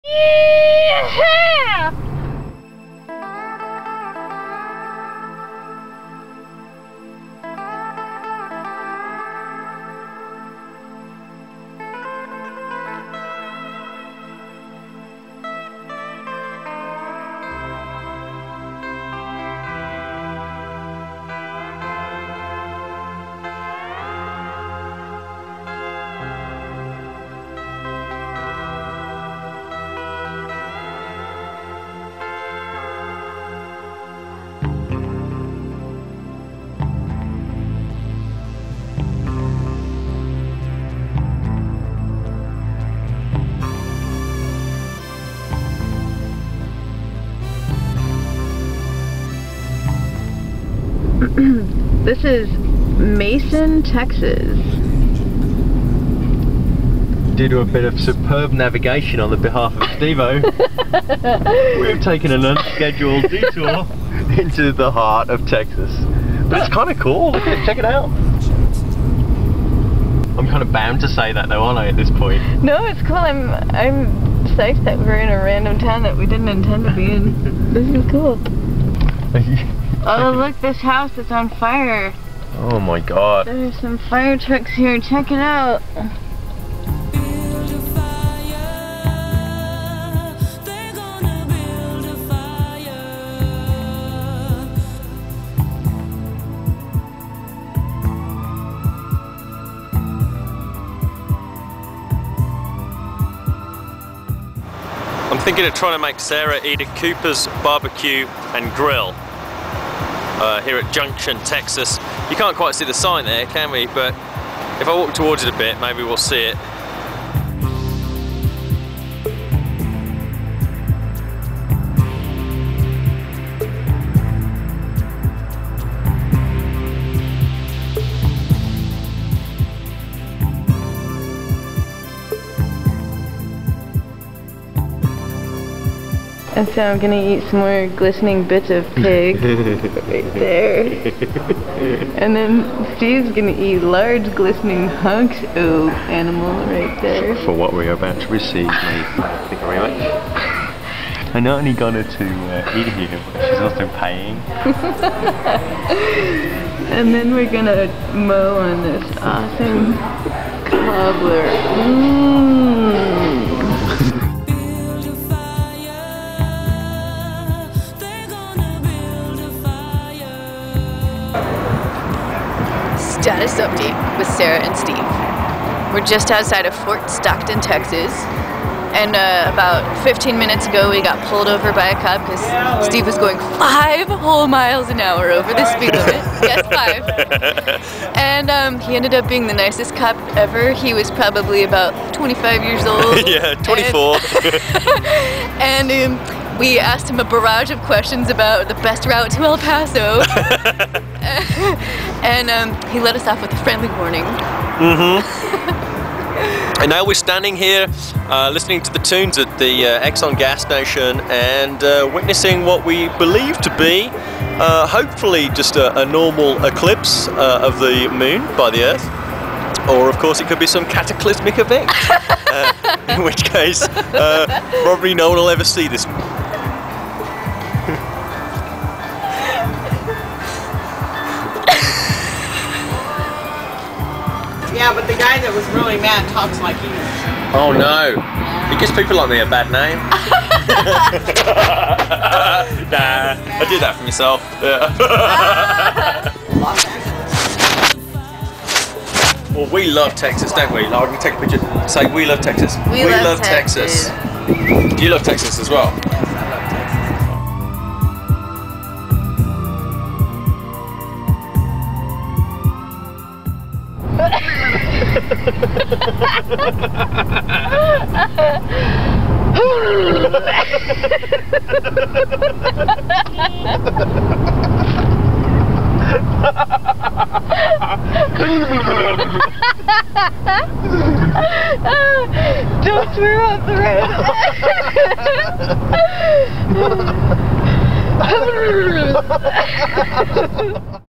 ROADTRIP day 06
Steve and Sarah go from Austin TX to Van Horn TX on the I-10 (plus a detour through Mason when we got lost).  Highlights include blackberry cobbler, getting pulled over, and the lunar eclipse.